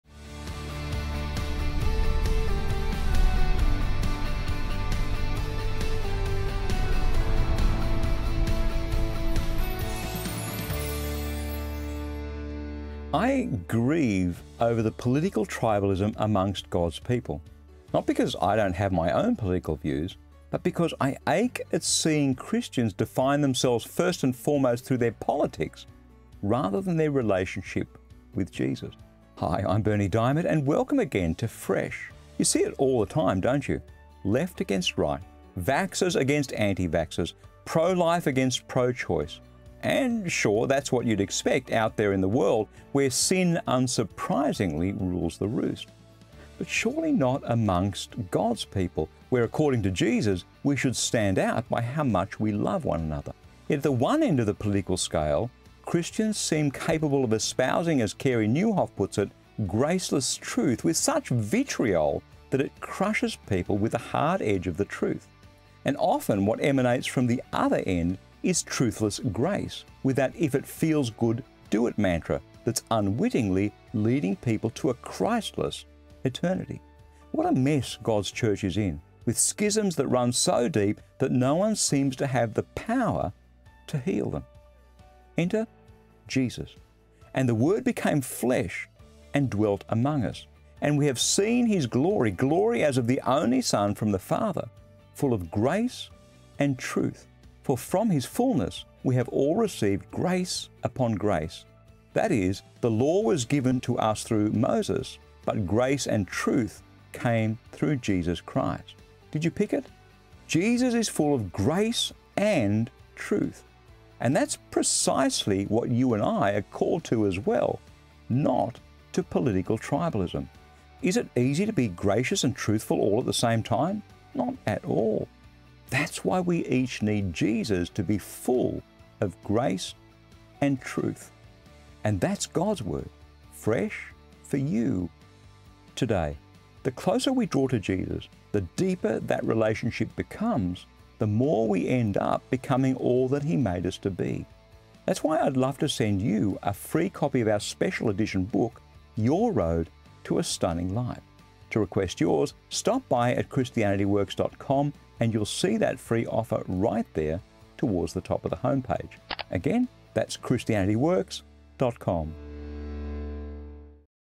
Both Grace and Truth
I grieve over political tribalism amongst God’s people. Not because I don’t have my own political views, but because I ache at seeing Christians define themselves first and foremost through their politics, rather than their relationship with Jesus.

You see it all the time, don’t you? Left against right. Vaxxers against anti-vaxxers. Pro-life against pro-choice. And sure, that’s what you expect out there in the world where sin, unsurprisingly, rules the roost. But surely not amongst God’s people, where according to Jesus, we should stand out by how much we love one another.

Yet at one end of the political scale, Christians seem capable of espousing, as Carey Nieuwhof puts it, graceless truth with such vitriol that it crushes people with the hard edge of the truth. And often what emanates from the other end is a truthless grace, with that “if it feels good do it” mantra that’s unwittingly leading people to a Christless eternity.

What a mess God’s church is in, with schisms that run so deep that no one seems to have the power to heal them. Enter Jesus:

John 1:14,16,17 And the Word became flesh and dwelt among us, and we have seen his glory, glory as of the only Son from the Father, full of grace and truth … For from his fullness we have all received, grace upon grace. That is, the law was given to us through Moses, but grace and truth came through Jesus Christ. (NRSV)

Did you pick it? Jesus is full of grace and truth. And that’s precisely what you and I are called to as well – not political tribalism. Is it easy to be gracious and truthful all at the same time? Not at all! That’s why we each need Jesus, to be full of grace and truth.